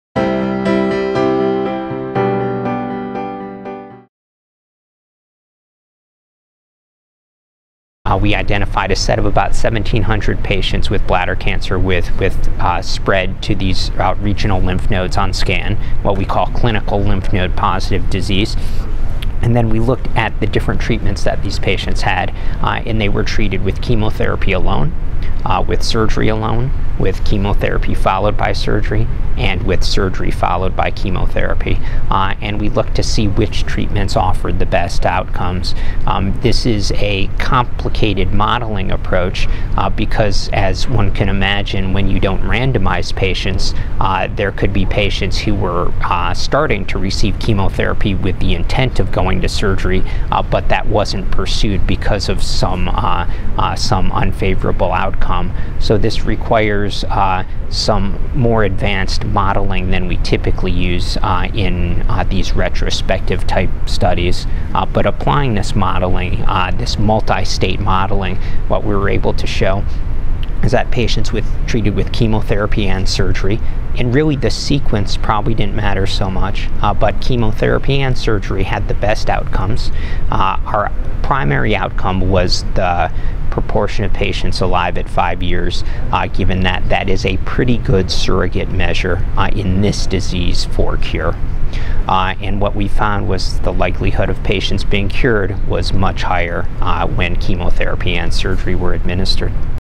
Matthew Galsky Discusses Treatment for Lymph Node-Positive Bladder Cancer
Matthew Galsky, MD, medical oncologist, Icahn School of Medicine at Mount Sinai, discusses treatment for lymph node-positive bladder cancer.